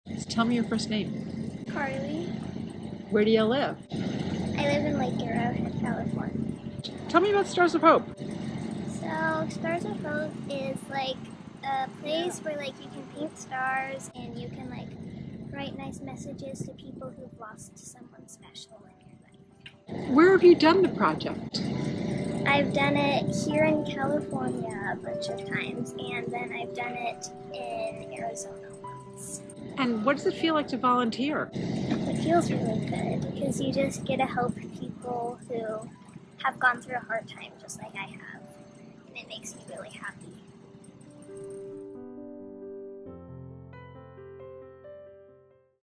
Karlie San Bernardino Volunteer
Young volunteers develop empathy, compassion, and kindness. Tools that last a lifetime.